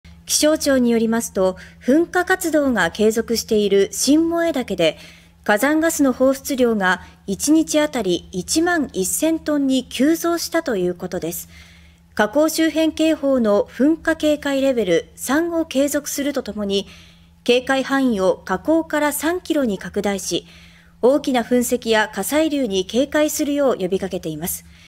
新燃岳の火山ガス急増 警戒範囲を火口から3キロに(17/10/15)
気象庁によりますと、噴火活動が継続している新燃岳で、火山ガスの放出量が一日あたり1万1000トンに急増したということです。火口周辺警報の噴火警戒レベル「3」を継続するとともに、警戒範囲を火口から3キロに拡大し、大きな噴石や火砕流に警戒するよう呼び掛けています。